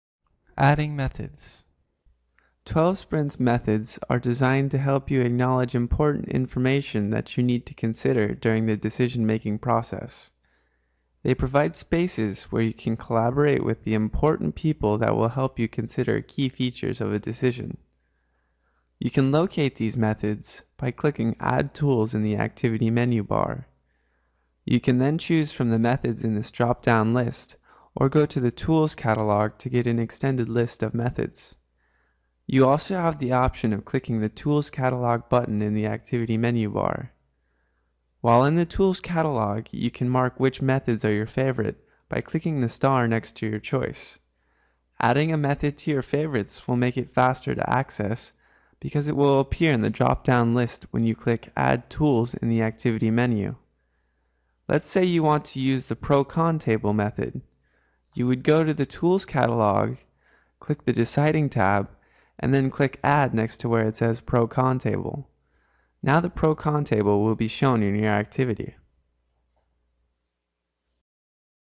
SAP 12sprints Tutorial: Adding Methods
SAP 12sprints: Learn how to add Decision Methods to your Activities.